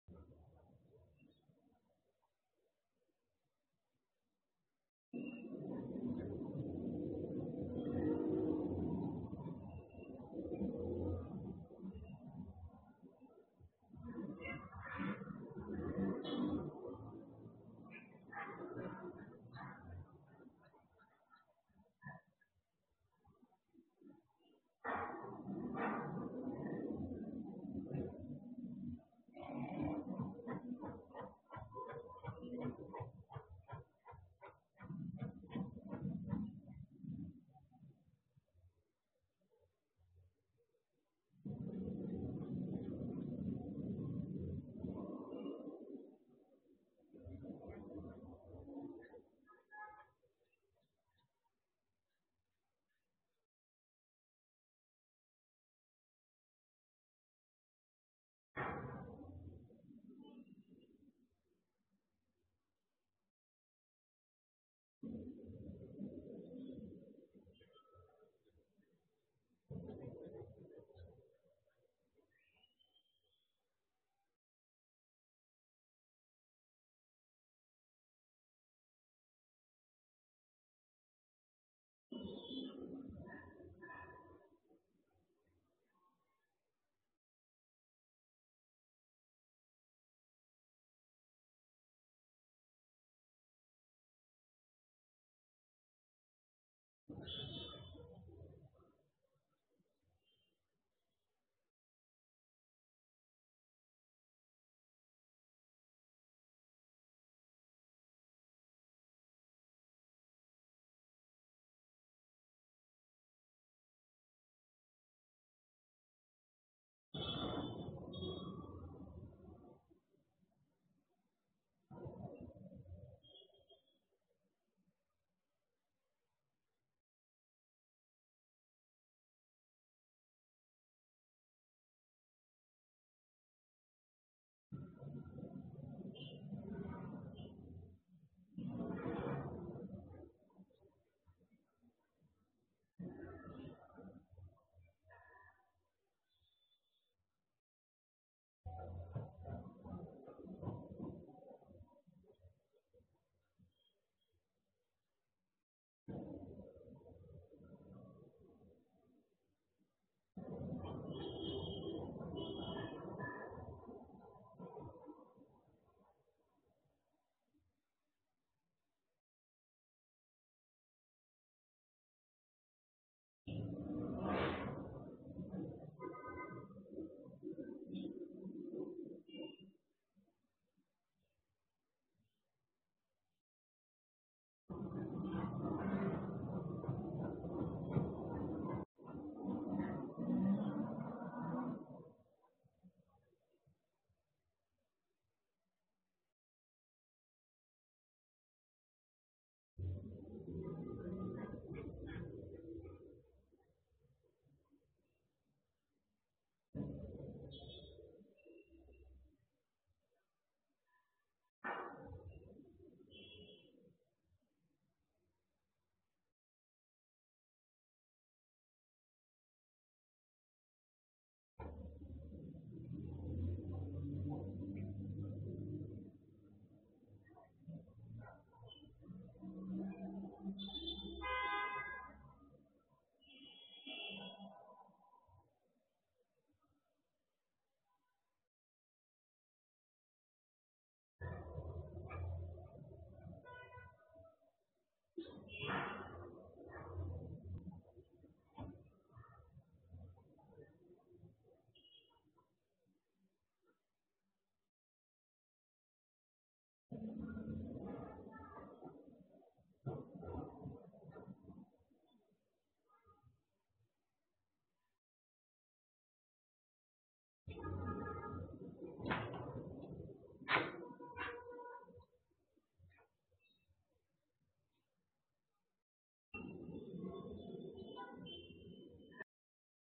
Beagle caught red-handed!
We finally found out what our beagle is doing when we leave her at home